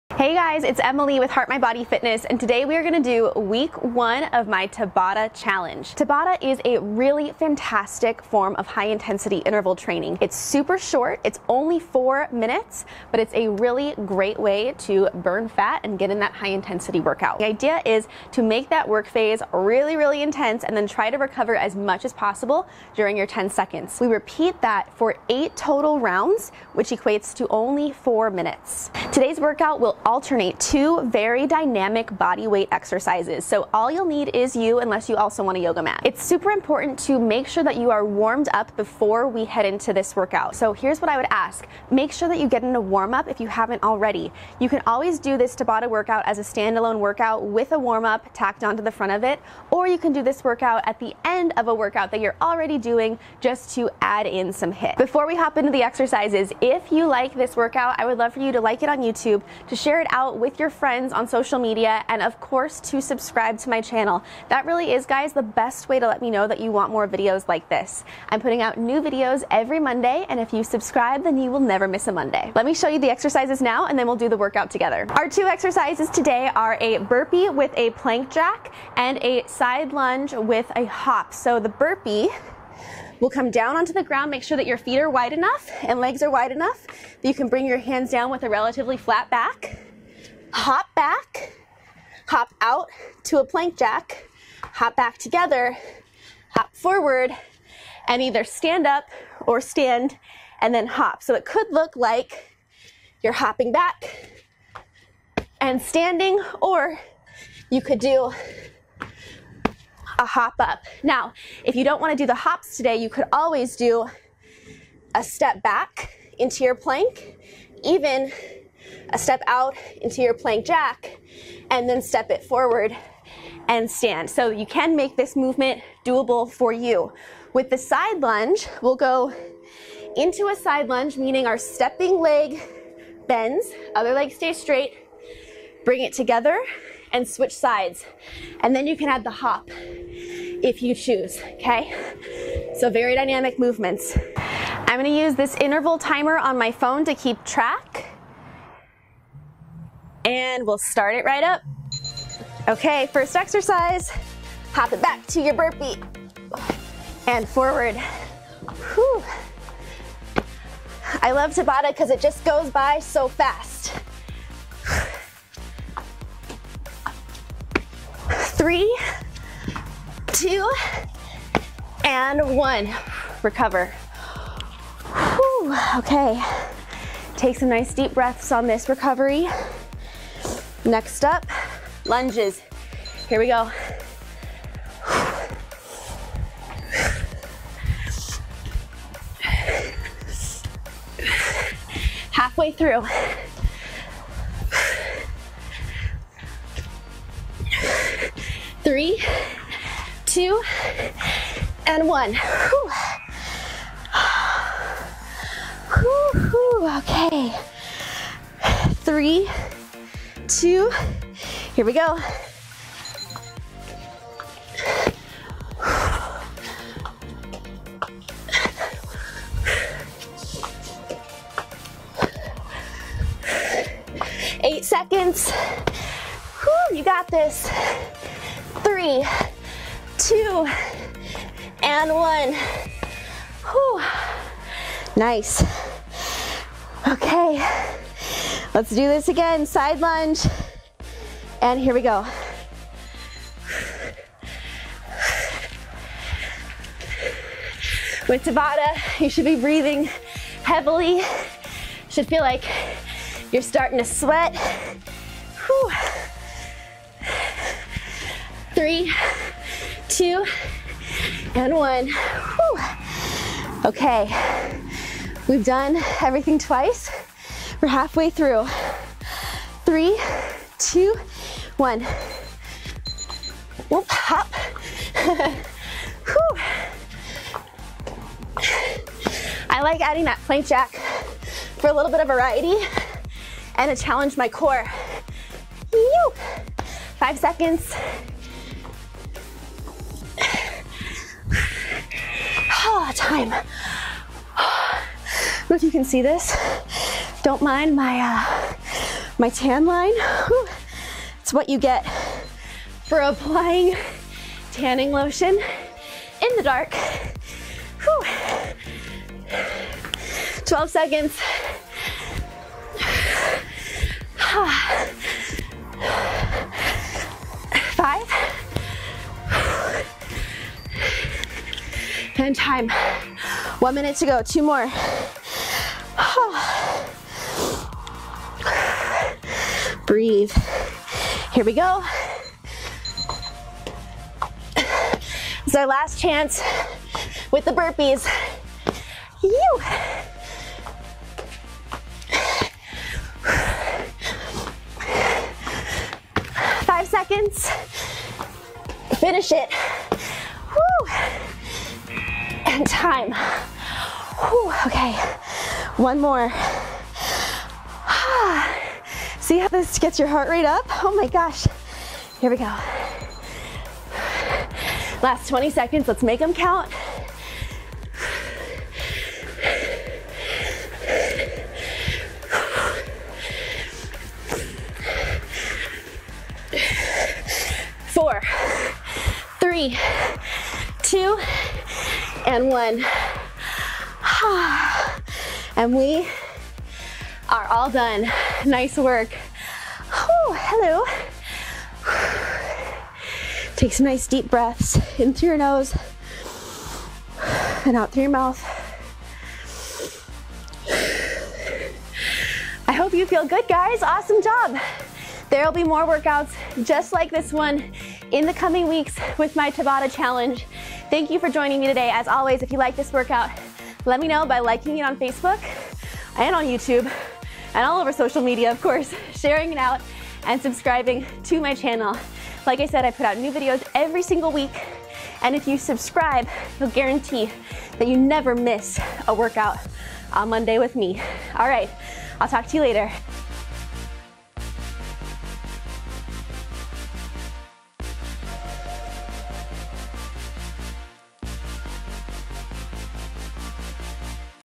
Bodyweight TABATA Workout | 4-Minute Tabata Workout at Home
Here is a bodyweight Tabata workout you can do at home in just 4 minutes! This intense full Tabata workout requires no equipment at all - just your bodyweight!

This workout is Week 1 of my 4-Week Tabata Challenge!